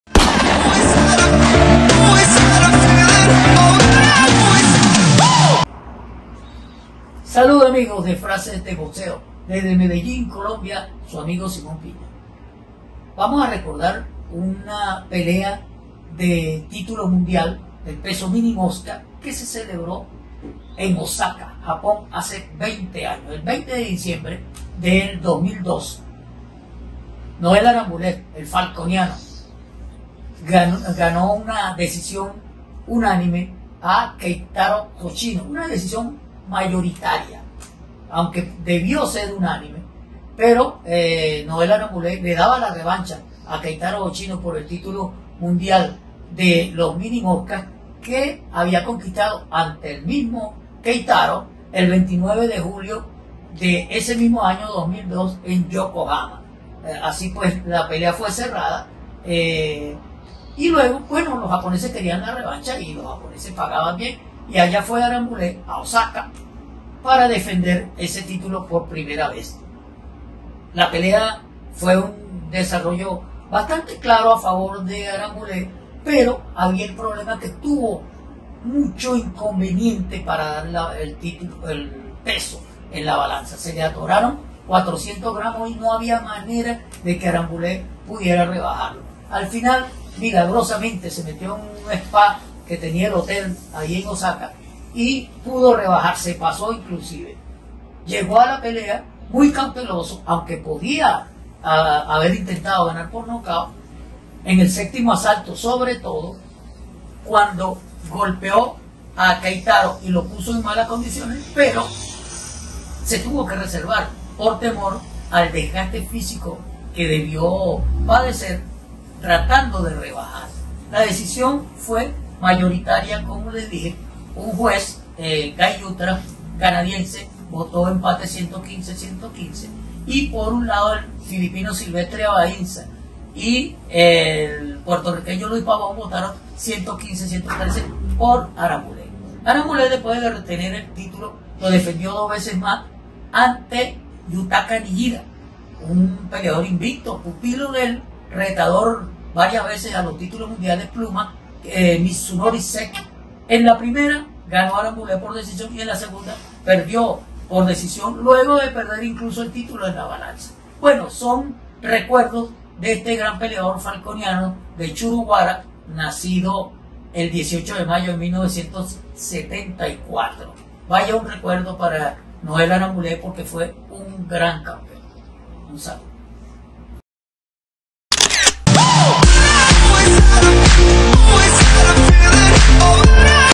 SIMON PIÑA CUENTA COMO NOEL ARAMBULET RETUVO TITULO MINI MOSCA ANTE KAITARO HOSHINO
El peleador venezolano Noel Arambulet, revalidó su título Mini Mosca AMB ante el campeón japonés Keitaro Hoshino al cual le había rebatado la corona el 29 de julio del 2002 en Yokohama, Japón .

El venezolano repitió su triunfo esta vez en Oxaca el 20 de diciembre del mismo año 2002, combate del cual se cumplieron 20 años esta semana.